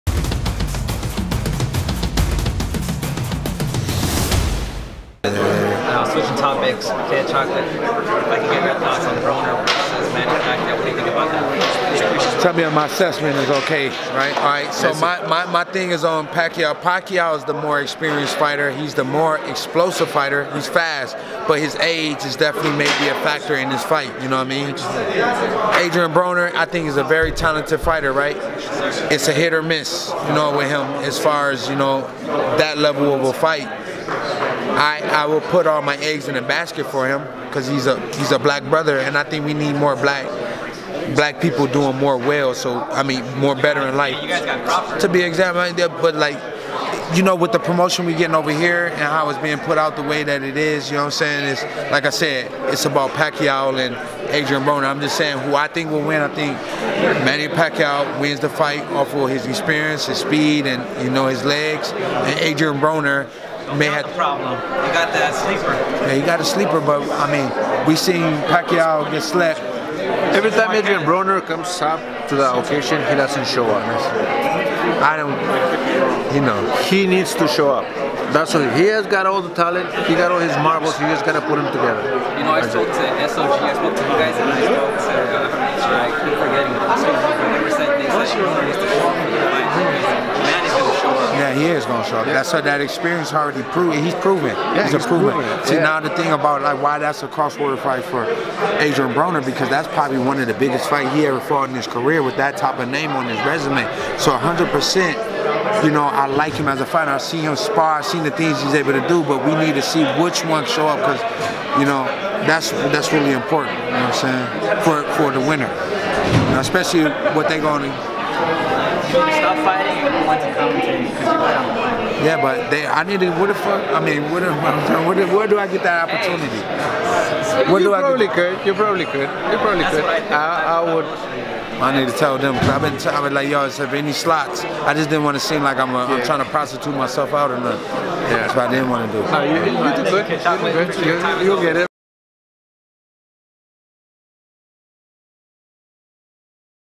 KID CHOCOLATE SAYS ADRIEN BRONER LOSES TO PACQUIAO, EXPLAINS WHY
In this video, Peter Quillin shares his thoughts on Manny Pacquiao vs Adrien Broner.